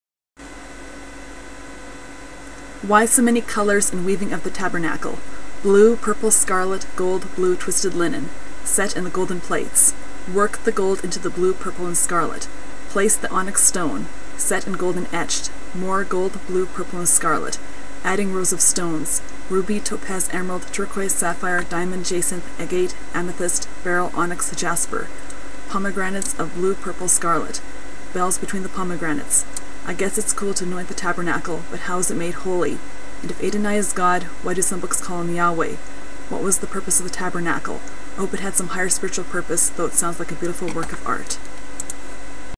A Rant On The Tabernacle
Why so many  colors and weaving of the tabernacle?
Blue, purple, scarlet, gold, blue, twisted linen.
Set in the golden plates.
Work the gold into the blue, purple and scarlet.
Place the onyx stone.
Set in gold and etched.
More gold, blue, purple and scarlet.
Adding rows of stones.
Ruby, topaz, emerald,
turquoise, sapphire,
diamond, jacinth,
agate, amethyst,
beryl, onyx, jasper.
Pomegranates of blue, purple, scarlet.
Bells between the pomegranates.
I guess it's cool to anoint the tabernacle,
but how is it made Holy?
And, if Adonai is God, why do some books call him Yahweh?
What was the purpose of the tabernacle?
I hope it had some higher spiritual purpose,
though it sounds like a beautiful work of art.